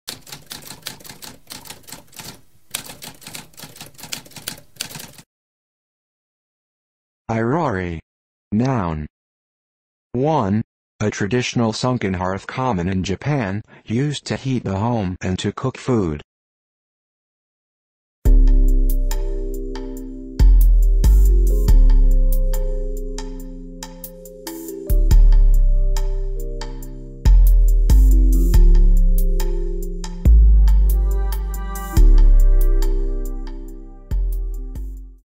What does irori mean?
A spoken definition of irori.

Intro Sound:
Typewriter - Tamskp